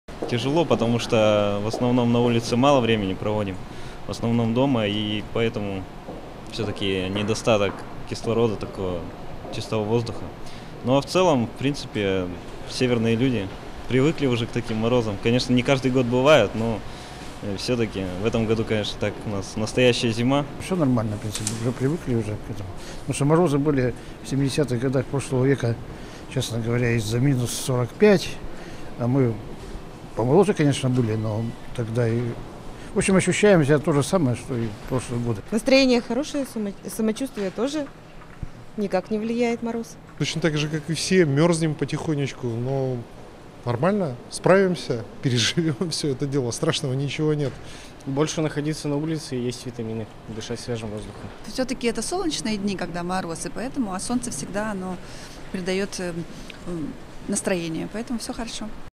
Вопрос дня - как вы переживаете морозы?
В Поморье стоит настоящая северная зима. На протяжении нескольких дней столбик термометра показывал выше 30 градусов мороза. Как архангелогородцы переносят суровые холода - в нашем традиционном опросе.